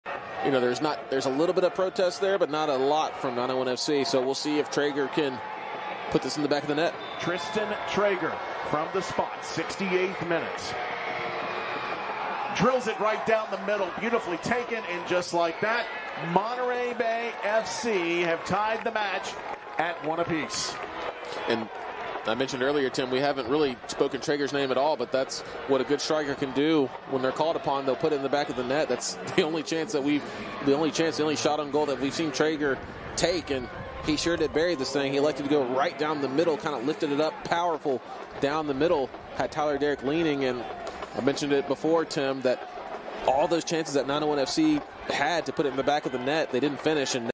Tristan Trager with a Penalty Goal vs. Memphis 901 FC
Tristan Trager (Monterey Bay F.C.) with a Penalty Goal vs. Memphis 901 FC, 04/20/2024